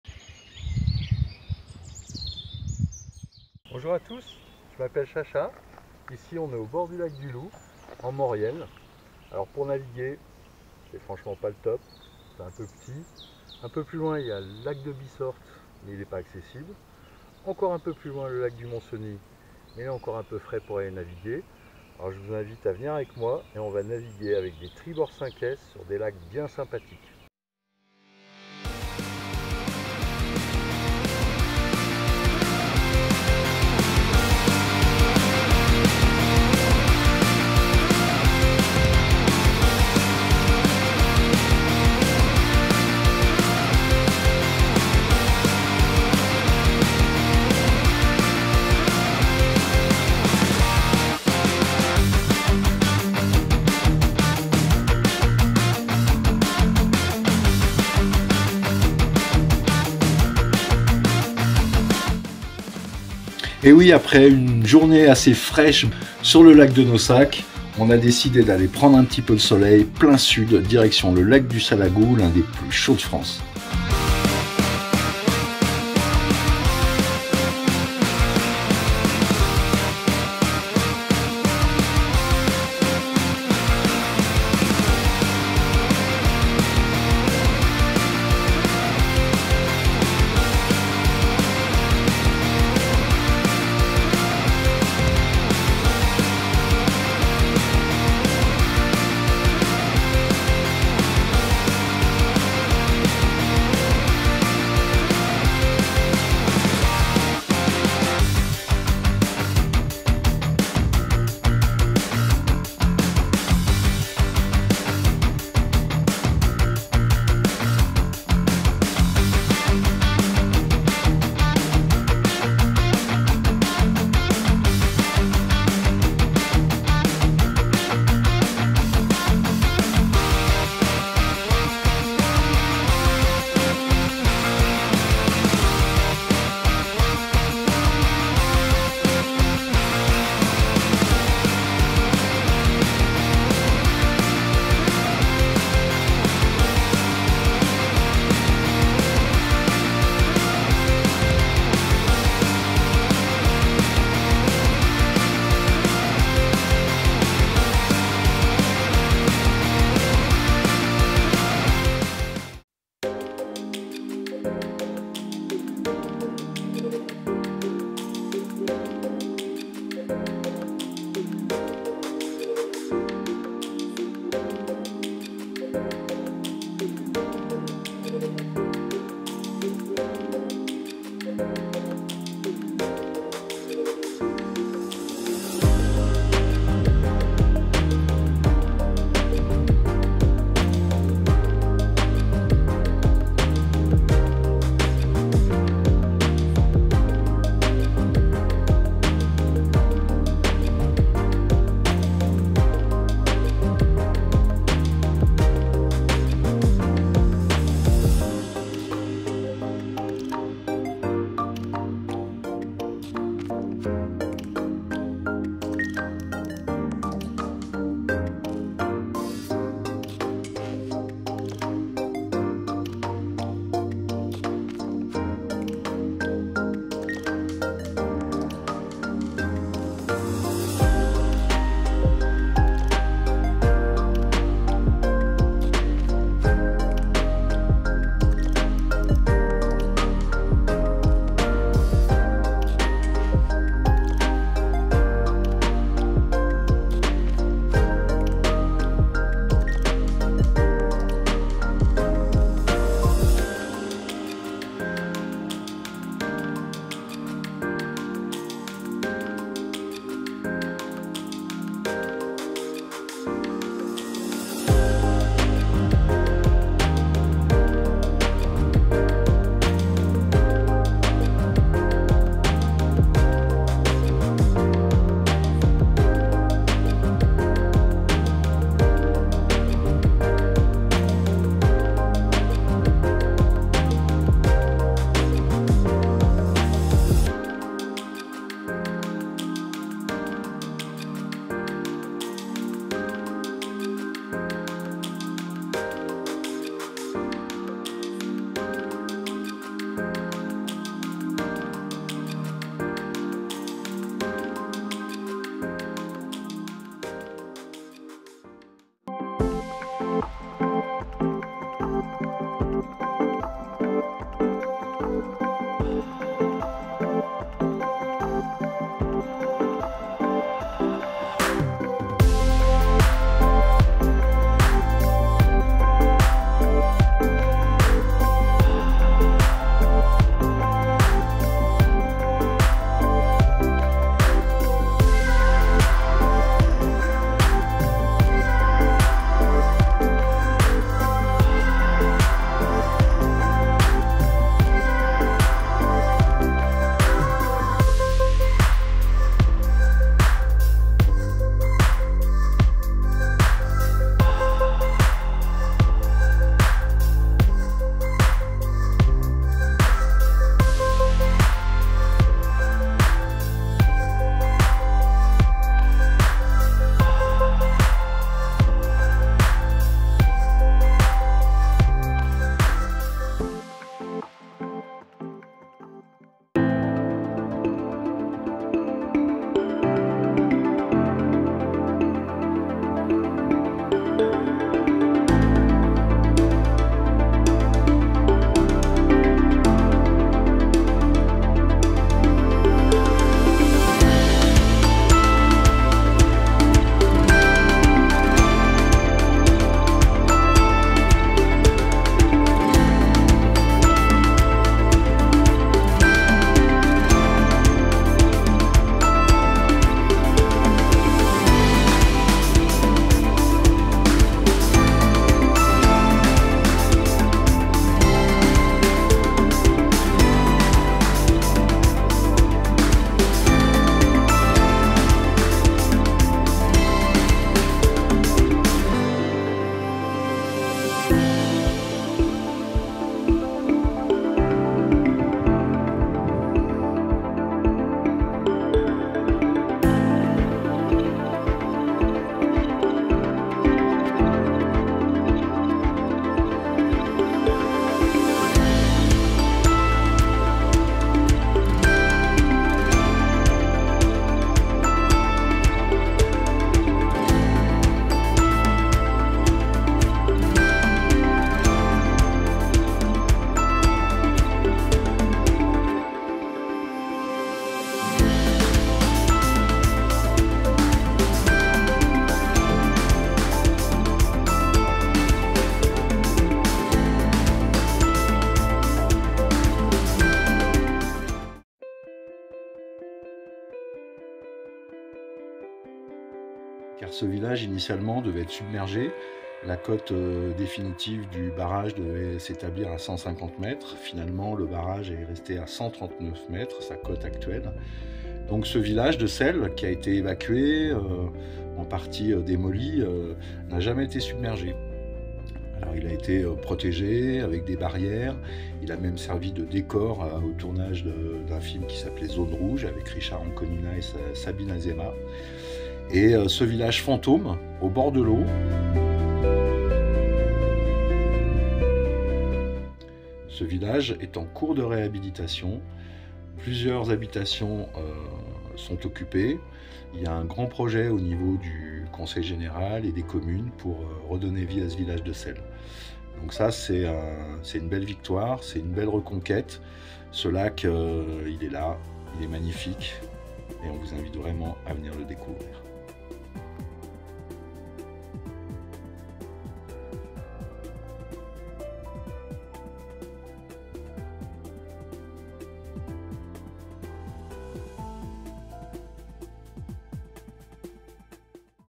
Lac de Salagou en Tribord 5S
Découverte en dériveur gonflable des lacs et plans d'eau